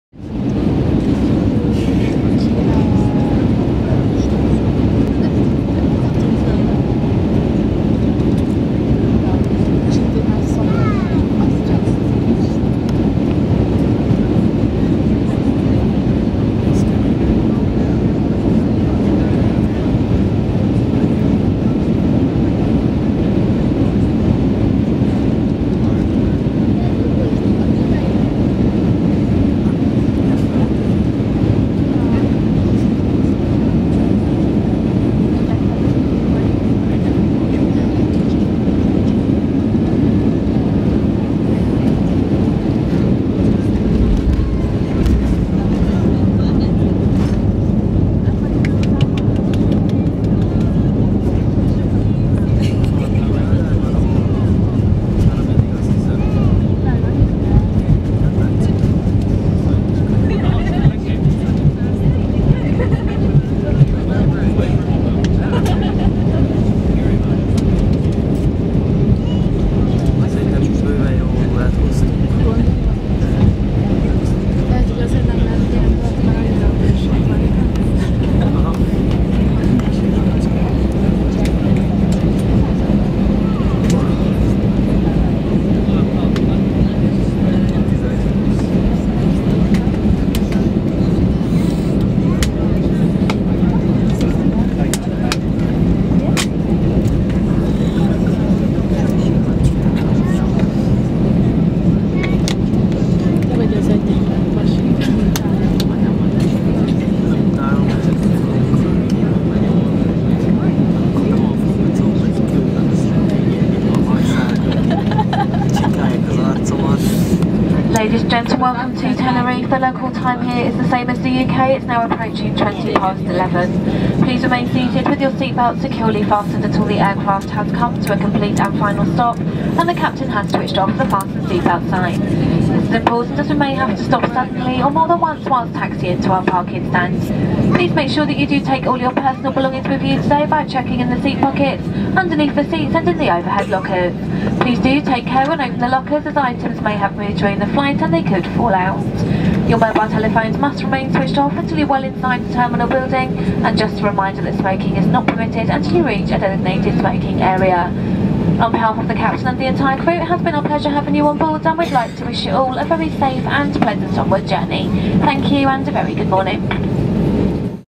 Landing at Tenerife airport
Easyjet Airbus320 Landing at Tenerife south. The flight from London Gatwick. Landing  11:05 local time. Flight lasted 4hr and 2 min. Manual Landing.